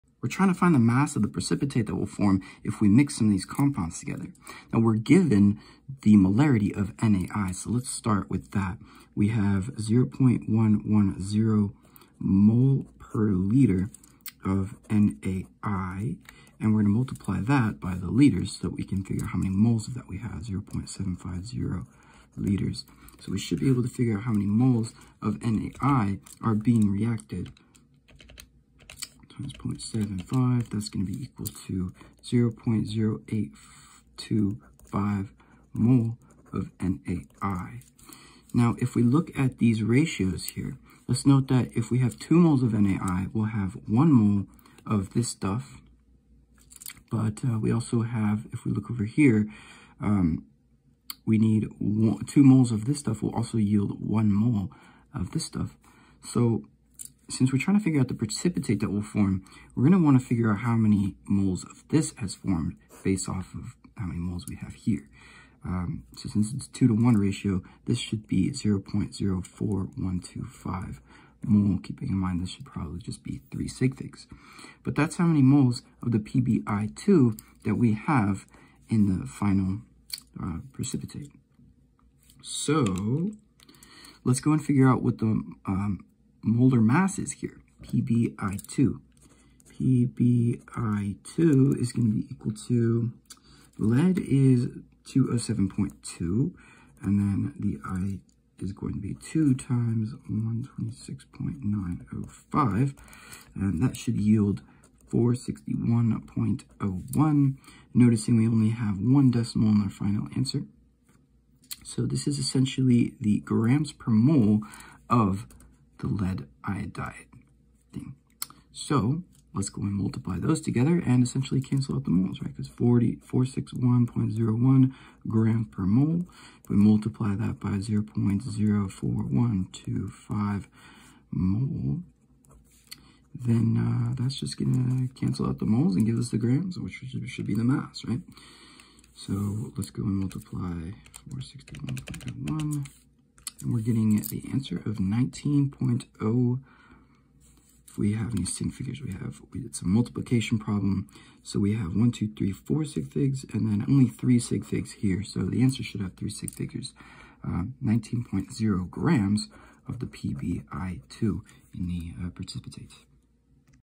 What mass of precipitate will form if highly concentrated Pb(ClO3)2 is mixed with NaI?
What mass of precipitate will form if highly concentrated Pb(ClO3)2 is mixed with NaI? Assume the reaction goes to completion.

Within the confines of her laboratory, Chloe embarks on an enthralling venture to demystify the interactions between lead(II) chlorate, Pb(ClO3)2, and sodium iodide, NaI, focusing on the captivating process of precipitation. This experiment, poised at the intersection of theory and tangible results, promises not just to validate stoichiometric predictions but to visually manifest the elegance of chemical reactions. As she meticulously combines the reactants, a dense precipitate emerges, from the collection of ions that results in this solid formation. This moment is not merely scientific procedure for Chloe; it's a testament to the allure of chemistry, where the blending of solutions transcends into the realm of discovery. The lab around her, cluttered with the tools of her trade—beakers, pipettes, and balances—stands as a testament to the rigorous yet creative spirit that defines her approach to science.